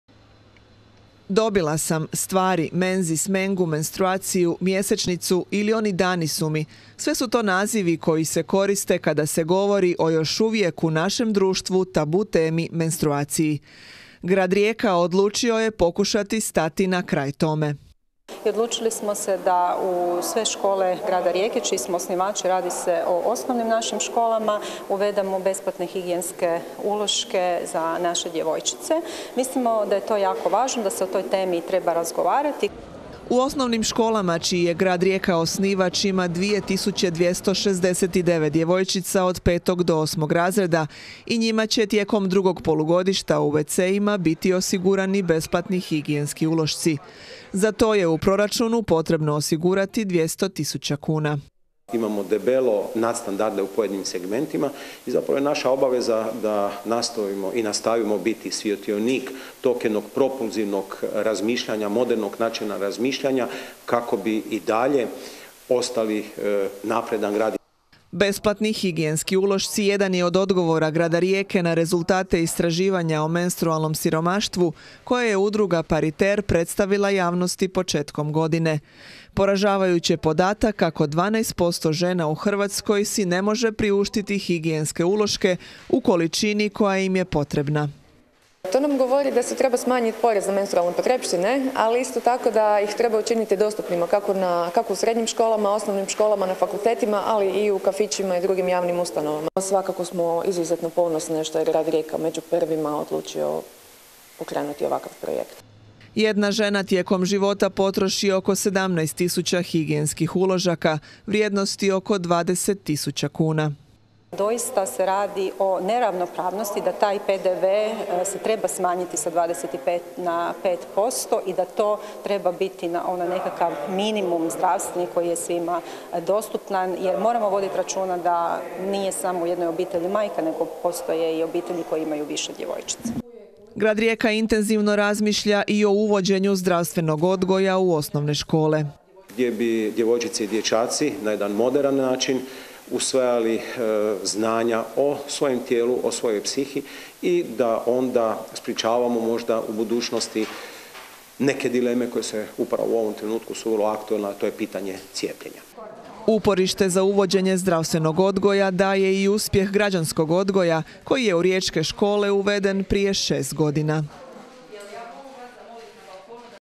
Rijeka uvela besplatne uloške u osnovne škole
Vlada odbila sniziti PDV na higijenske uloške, a Rijeka će ih od sljedećeg polugodišta besplatno omogućiti svim gradskim osnovnim školama.